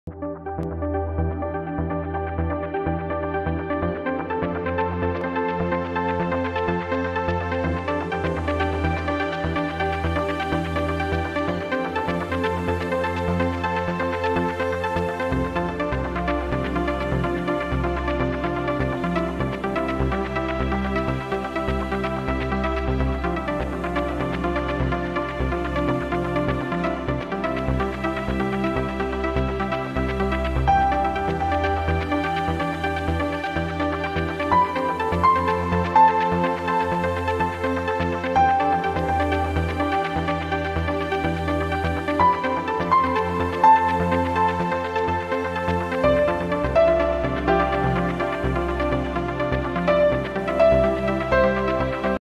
Virtualus Dangvietų turas / Virtual Tour of Dangvietai, Lithuania
pasivažinėjimas po Dangvietus (Kelmės raj.) / Virtual Tour of Dangvietai, Lithuania / Виртуальный тур по Дангвиетай, Литва / Tour virtuel de Dangvietai, Lituanie / Virtuelle Tour durch Dangvietai, Litauen